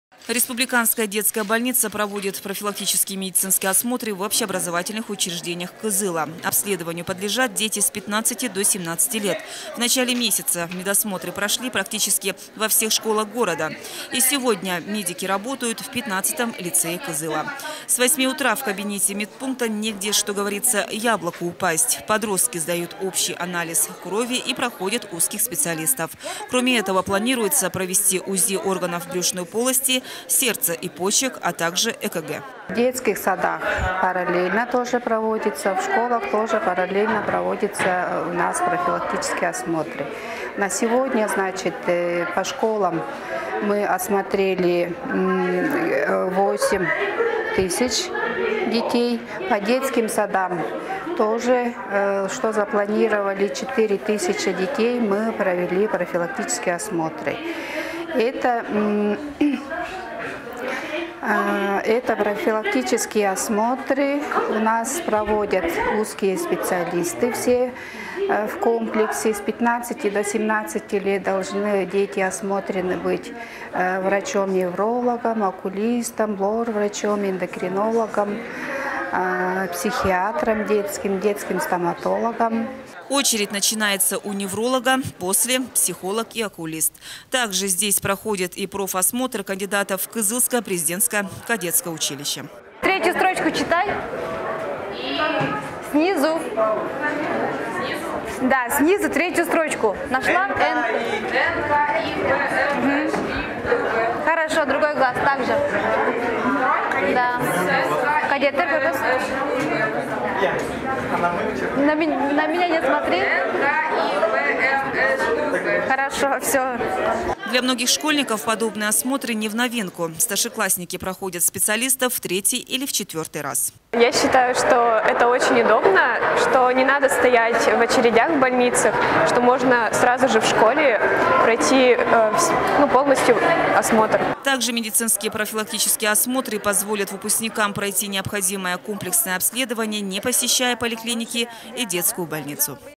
Республиканская детская больница проводит профилактические медицинские осмотры
Обследованию подлежат дети с 15 до 17 лет. В начале месяца медосмотры прошли практически во всех школах города. И сегодня медики работают в 15-м лицее Кызыла. С восьми утра в кабинете медпункта негде, что говорится, яблоку упасть. Подростки сдают общий анализ крови и проходят узких специалистов. Кроме этого планируется провести УЗИ органов брюшной полости, сердца и почек, а также ЭКГ.